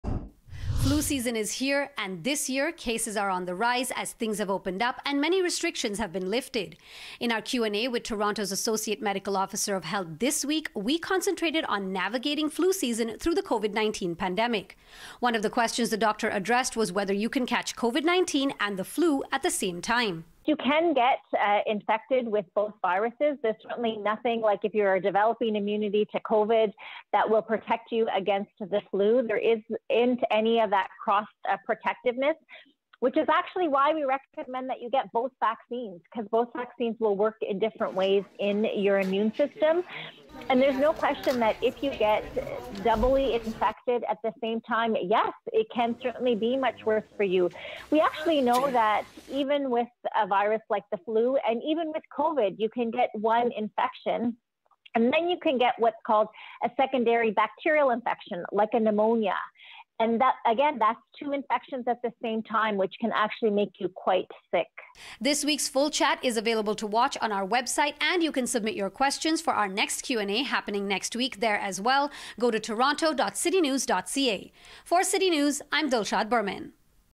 Coronavirus Q&A: Can you get COVID-19 and the flu at the same time?
In our Q&A with Toronto’s Associate Medical officer of Health, this week, we focused on navigating flu season through the COVID-19 pandemic. One of the questions the doctor addressed was whether you can catch COVID-19 and the flu together.